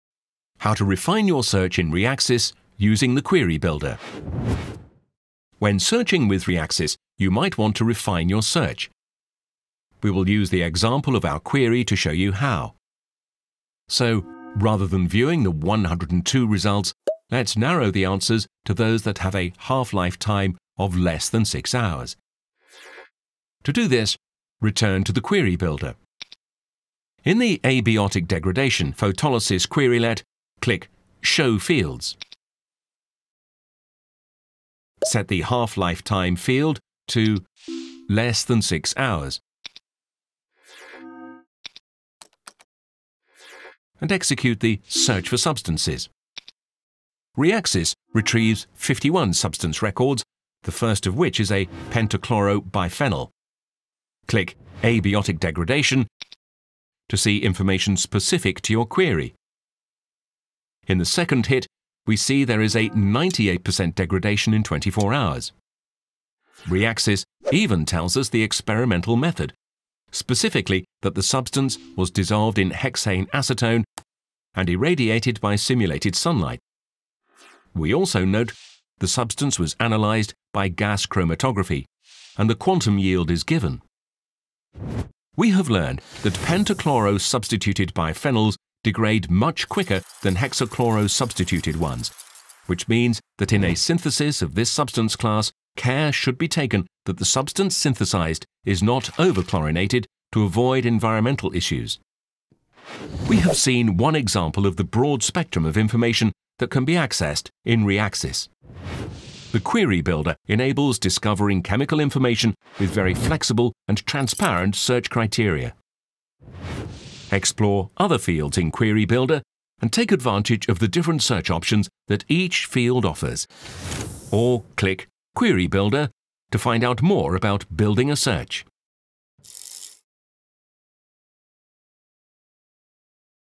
Learn Reaxys : How to refine a search using Query Builder in Reaxys
This video will teach you how to find information on compound properties by using Quick Builder.

Does your institution have Reaxys? Then start your learning journey! Saty up to date with Reaxys! Would you like to learn Reaxys form our experts?

Thanks to the RxEd Program team: Paloma Engel Garcia, Marta Da Pian, Eleonora Echegaray, Ryan Liffey, Alica May, Piotr Gołkiewicz, Friedrich Kroll, Paola Rosciano Balmori